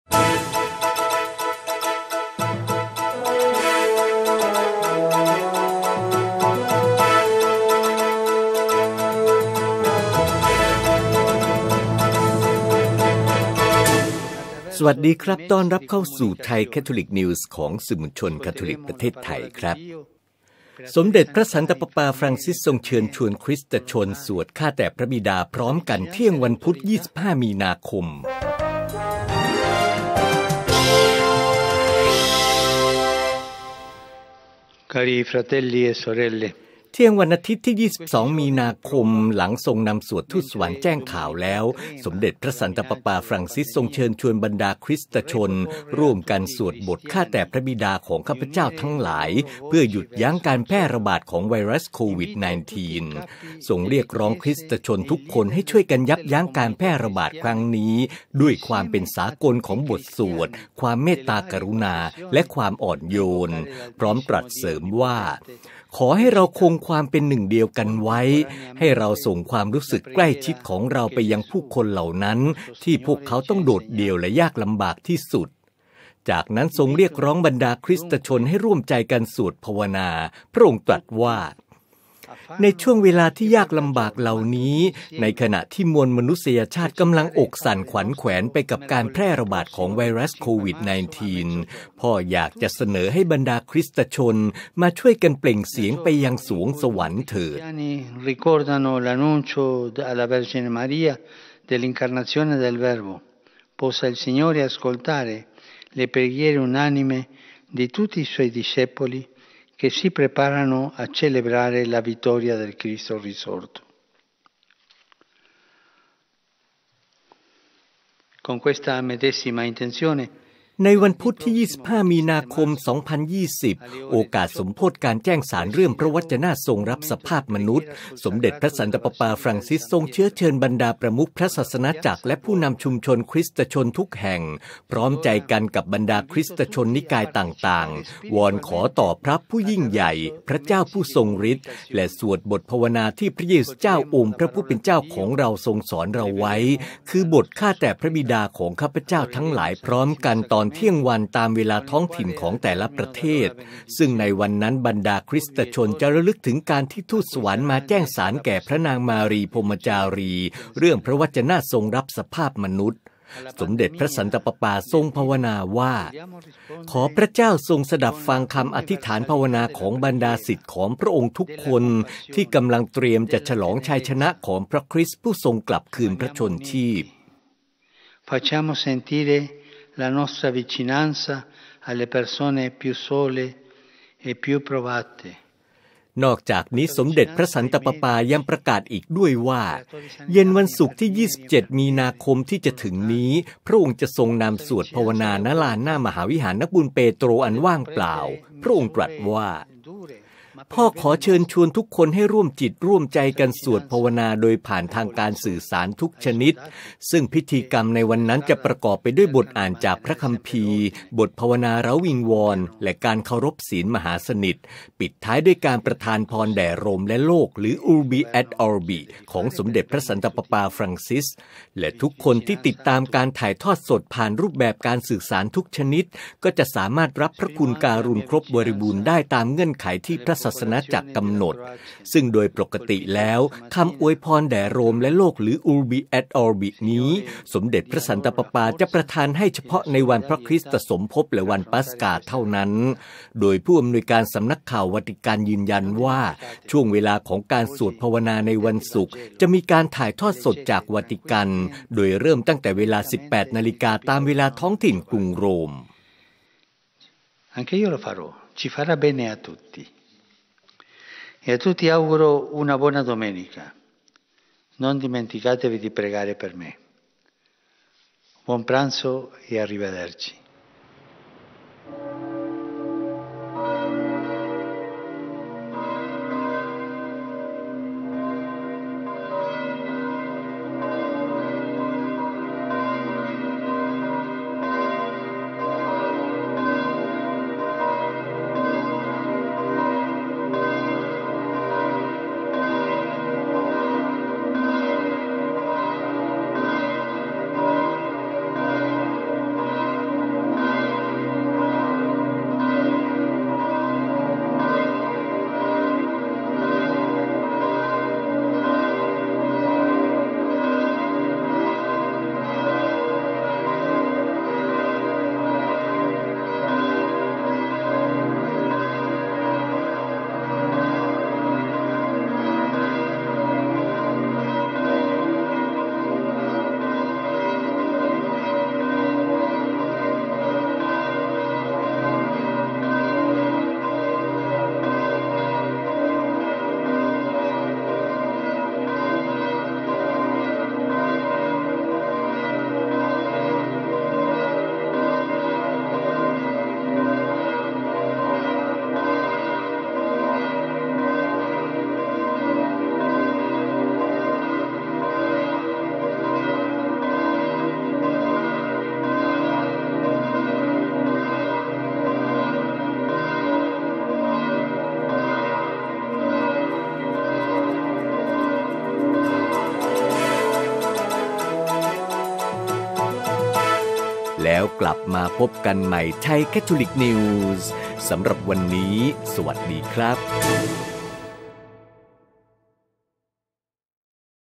ข่าวพระศาสนจักรคาทอลิก 22-03-2020 โป๊ปนัดสวดพร้อมกัน “เที่ยงวันพุธ” (25 มี.ค. 2020)
เที่ยงวันอาทิตย์ที่ 22 มีนาคม 2020 หลังทรงนำสวดทูตสวรรค์แจ้งข่าวแล้ว สมเด็จพระสันตะปาปาฟรังซิสทรงเชิญชวนบรรดาคริสตชนร่วมกันสวดบท “ข้าแต่พระบิดาของข้าพเจ้าทั้งหลาย” พร้อมกันตอนเที่ยงวันพุธที่ 25 มีนาคม 2020 นี้ เพื่อหยุดยั้งการแพร่ระบาดของไวรัสโควิด-19